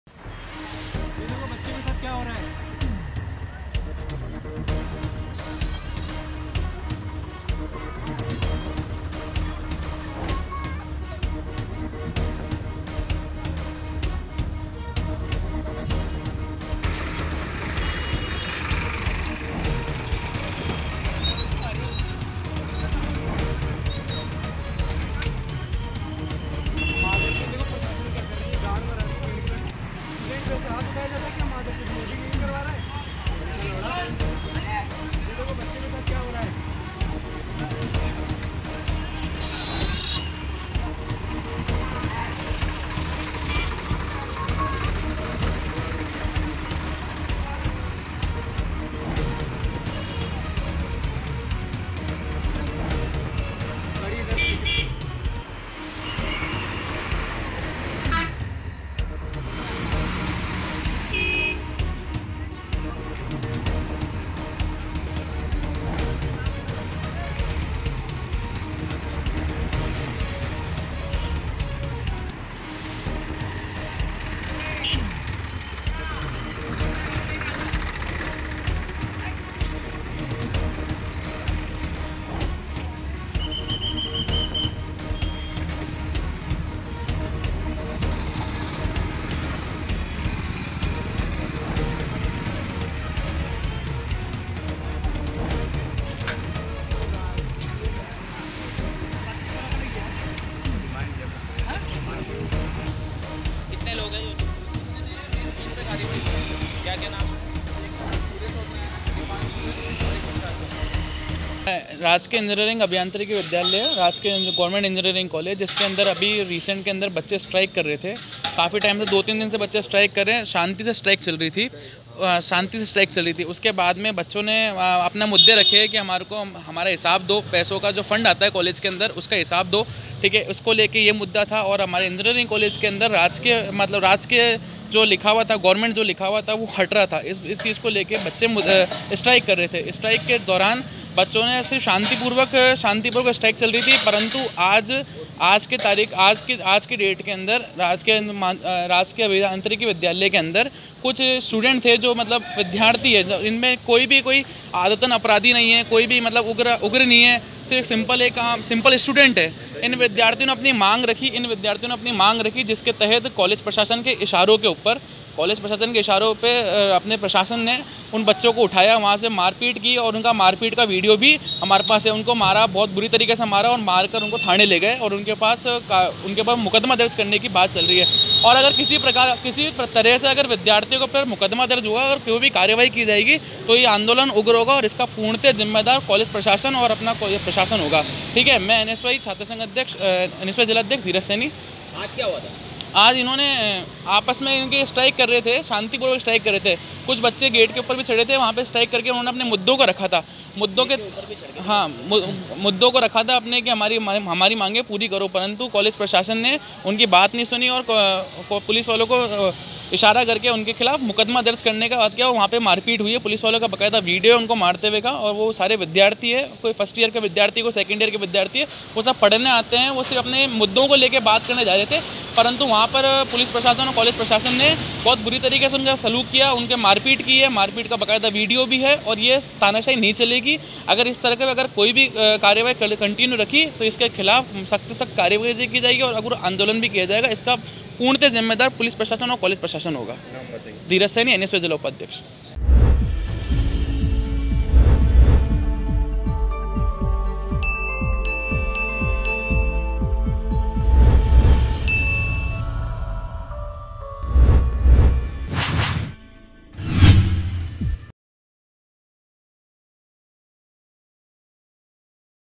AJMER NEWS | राजकीय शब्द हटाने का मामला | MTTV INDIA
राजकीय शब्द हटाने का मामला
राजकीय इंजीनियरिंग कॉलेज अजमेर में छात्र का हंगामा
इमारत पर चढ़कर छात्रों ने किया विरोध प्रदर्शन
छात्र को समझाने के किए गए प्रयास
सूर्य चौधरी बताया जा रहा है छात्र का नाम
अलवर गेट थाना पुलिस ने समझाइश के बाद छात्र को उतारा नीचे